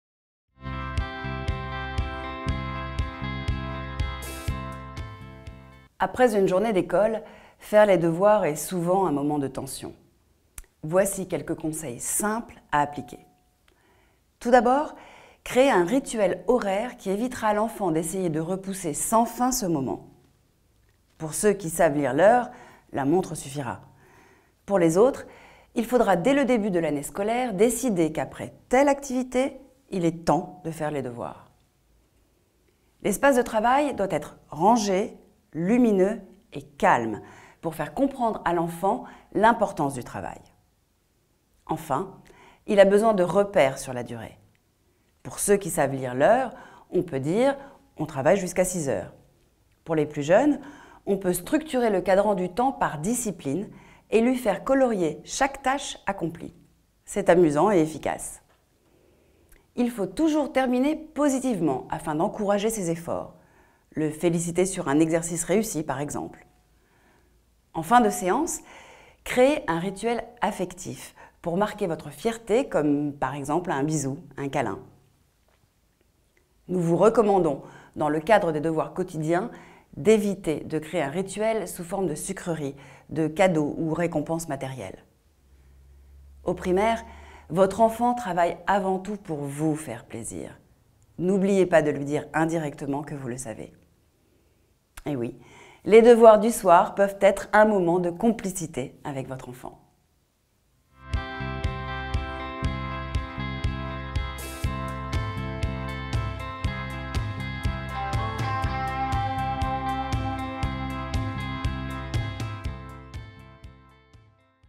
Comment aider mon enfant à faire ses devoirs à la maison
Après une journée d’école, faire les devoirs est souvent un moment de tension…
Voici quelques conseils simples à appliquer.

Tout d’abord, créer un rituel horaire qui évitera à l’enfant d’essayer de repousser sans fin ce moment…

Pour ceux qui savent lire l’heure, la montre suffira.

Pour les autres, il faudra dès le début de l’année scolaire décider qu’après telle activité, il est temps de faire les devoirs.

L’espace de travail doit être rangé, lumineux et calme pour faire comprendre à l’enfant l’importance du travail.

Enfin, il a besoin de repères sur la durée.

Pour ceux qui savent lire l’heure, on peut dire « on travaille jusqu’à six heures »…
Pour les plus jeunes, on peut structurer le cadran du temps par discipline et lui faire colorier chaque tâche accomplie. C’est amusant et efficace.

Il faut toujours terminer positivement afin d’encourager ses efforts. Le féliciter sur un exercice réussi par exemple.

En fin de séance, créez un rituel affectif pour marquer votre fierté comme par exemple un bisou sur le front.

Nous vous recommandons dans le cadre des devoirs quotidiens, d’éviter de créer un rituel sous forme de sucrerie, de cadeau ou récompense matérielle.

Au primaire, votre enfant travaille avant tout pour VOUS faire plaisir ! N’oubliez pas de lui dire indirectement que vous le savez…

Eh oui, les devoirs du soir peuvent être un moment de complicité avec votre enfant !